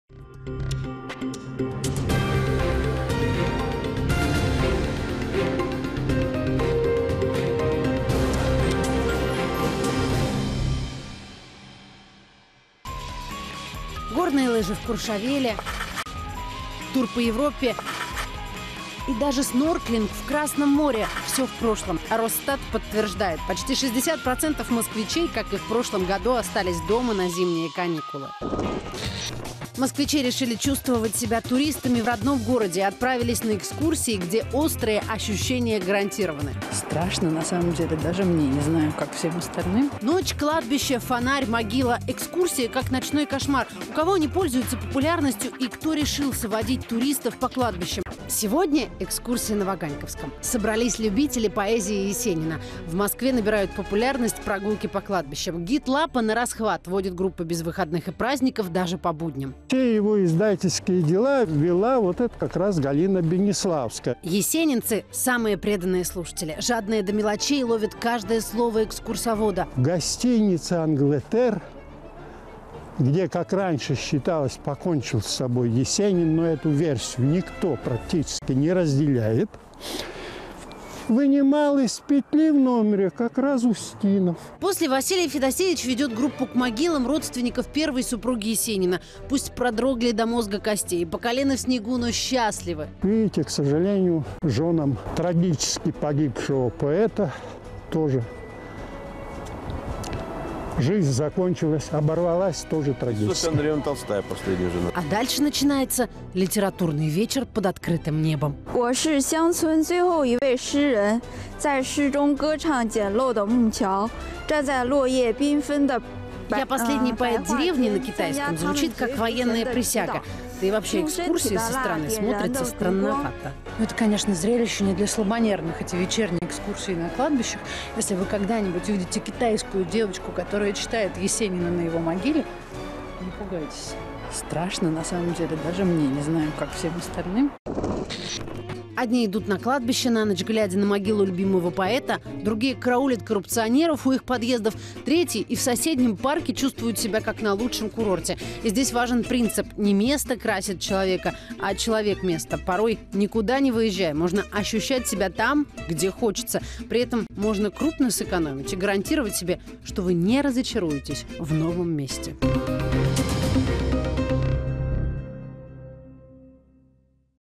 Экскурсии по местам захоронений известных людей.
Да, Василий Федосеевич Лапа пишет книги и водит вдохновенные экскурсии по знаменитым кладбищам Москвы для ценителей нашей истории. Там похоронено много известных людей. За каждым из них история, порой интересная и загадочная. И вовсе не ночью. Просто зимой рано темнеет. Но как это подано! Прям, ужастик. А чего вы еще ждали от нашего телевидения? Они же любят делать инфоповод на пустом месте. Москва 24, стыдись!